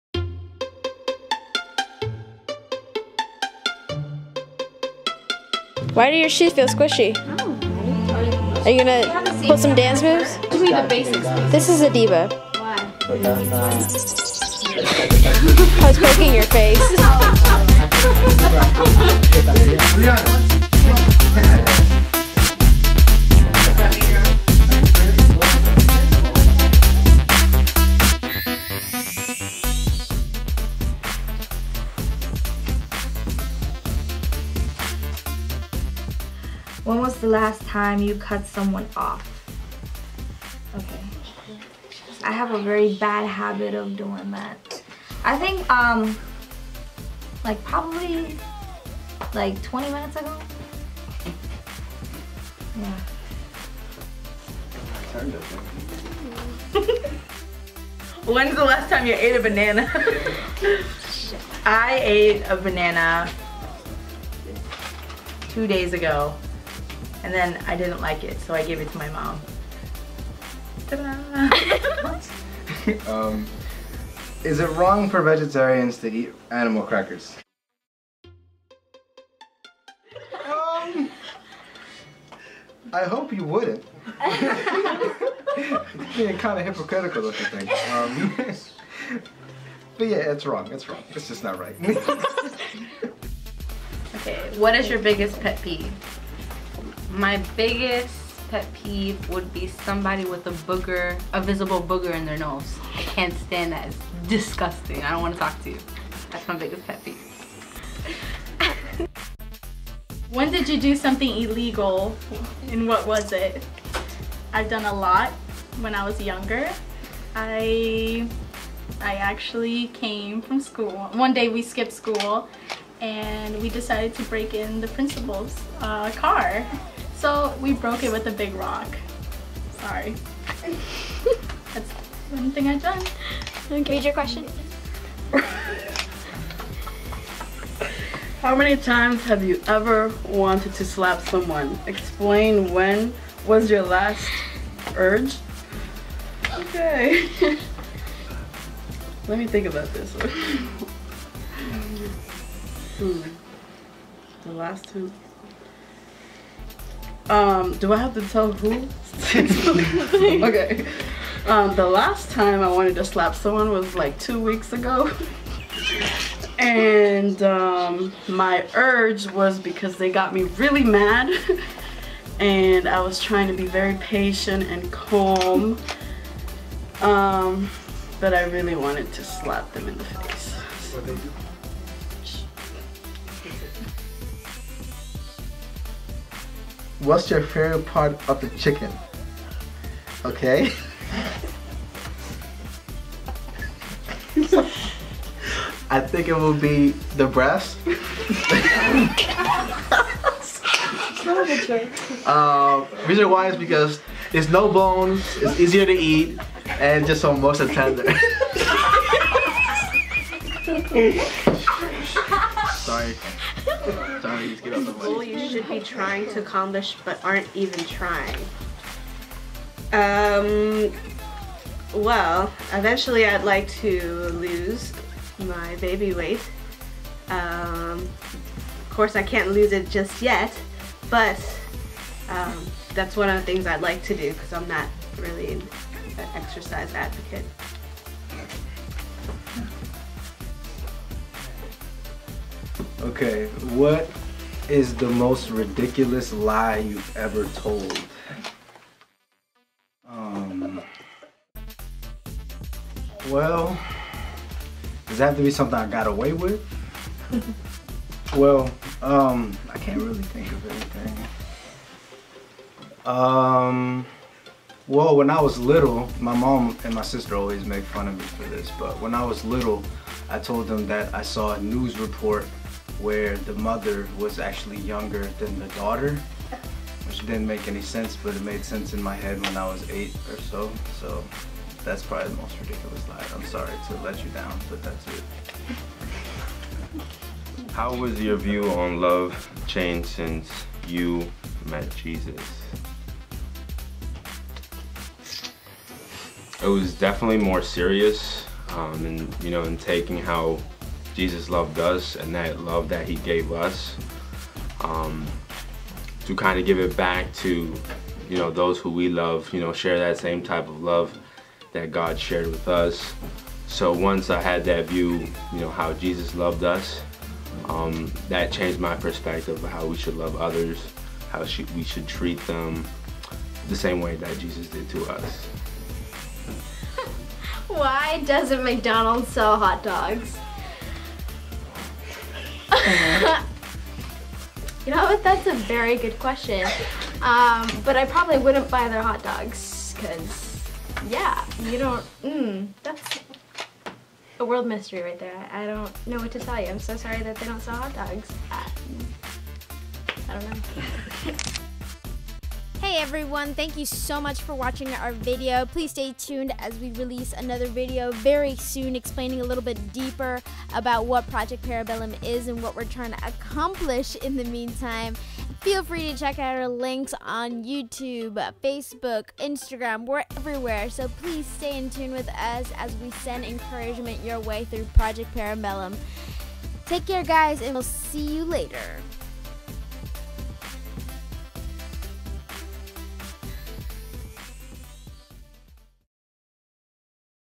Project Parabellum: Introduction
This is an introduction for Project Parabellum.
We want to bring positive impact on past, present, and future generations through social media by showing God's love.